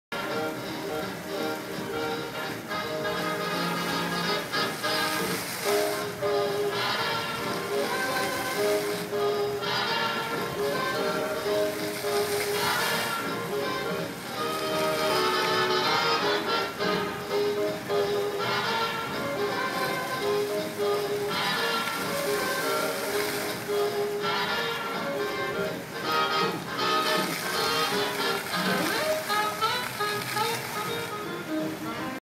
Fountains on the Waterway in The Woodlands
7/16/11 - Joyce and I took Jay and Rich on the water taxi, got off at Gooses Acre and then walked around some.